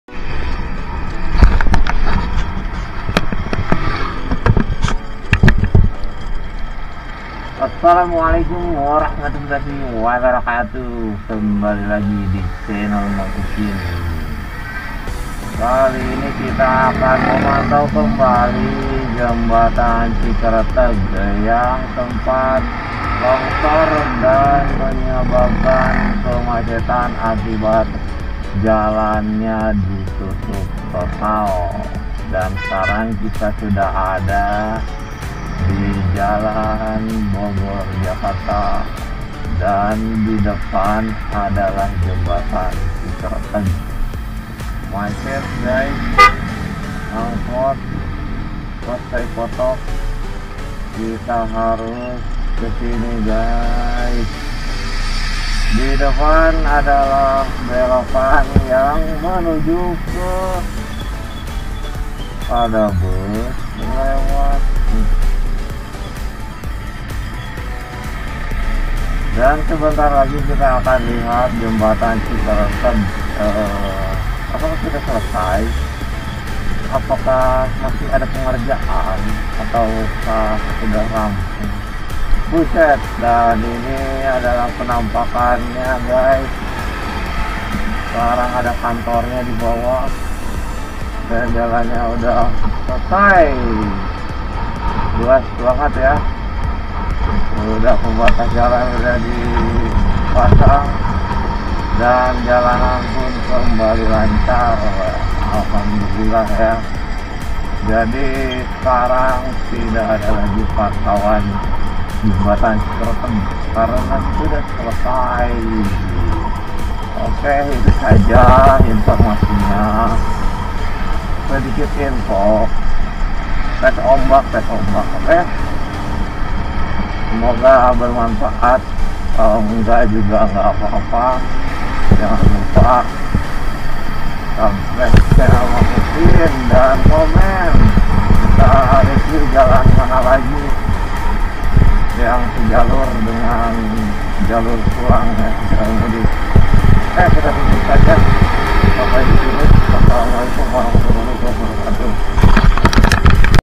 KEMACETAN DI JALAN SUKABUMI BOGOR BERKURANG | JEMBATAN CIKERETEG TERBARU
KEMACETAN DI JL SUKABUMI BOGOR BERKURANG | JEMBATAN CIKERETEG TERBARU

📷 SELASA 19 Des 2023
Alhamdulillah sedikit demi sedikit kemacetan di Jl sukabumi bogor berkurang, terutama di jembatan CIKERETEG, sekarang sudah lancar, tidak macet lagi.
Berikut adalah kondisi terbaru dari jembatan CIKERETEG terbaru.